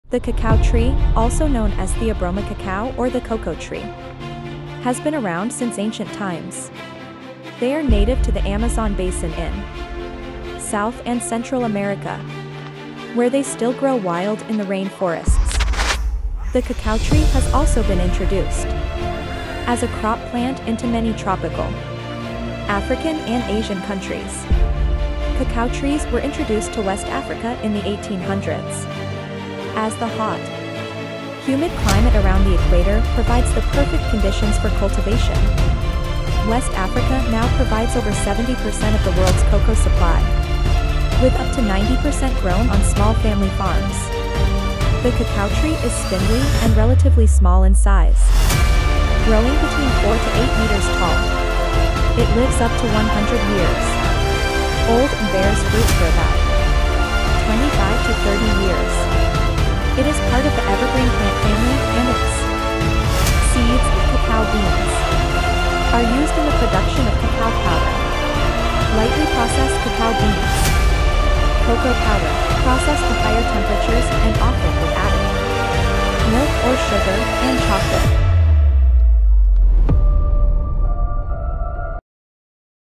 Chocolate tree / Coco tree
The cacao tree, also known as Theobroma Cacao or the cocoa tree, has been around since ancient times. They are native to the Amazon basin in South and Central America where they still grow wild in the rain-forests. The cacao tree has also been introduced as a crop plant into many tropical African and Asian countries.

Cacao trees were introduced to West Africa in the 1800s, as the hot, humid climate around the equator provides the perfect conditions for cultivation. West Africa now provides over 70% of the world’s cocoa supply, with up to 90% grown on small family farms.

The cacao tree is spindly and relatively small in size, growing between 4-8 metres tall. It lives up to 100 years old and bears fruit for about 25-30 years. It is part of the evergreen plant family and its seeds (cacao beans) are used in the production of cacao powder (lightly processed cacao beans), cocoa powder (processed to higher temperatures and often with added milk or sugar) and chocolate.